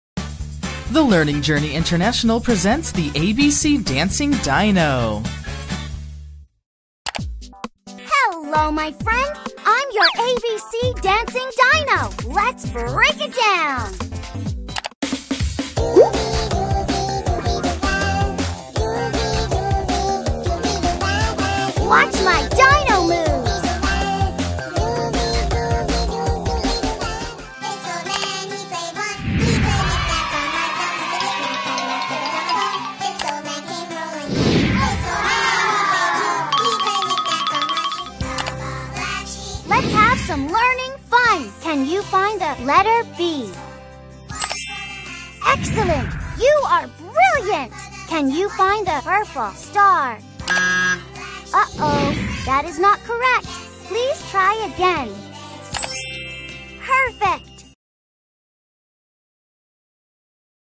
The Learning Journey Remote Control ABC Dancing Dino
Kids are riveted by this delightfully interactive educational toy. Remote Control ABC Dancing Dino responds to the touch of various buttons on the accompanying handheld remote: walking and talking, dancing, singing, and playing music. He even asks questions and confirms whether kids have selected the correct answer. His wiggly, waggly tail and enthusiastic dance moves (accompanied by humorous phrases like 'I'm a prehistoric dancing machine!'), ignite particularly gleeful responses from young ones. Fun songs and sound effects keep children happily engaged, laughing, dancing, and singing along to familiar tunes. Between Action Mode and Let's Learn Mode (both of which are triggered by remote control) ABC Dancing Dino covers a lot of play territory.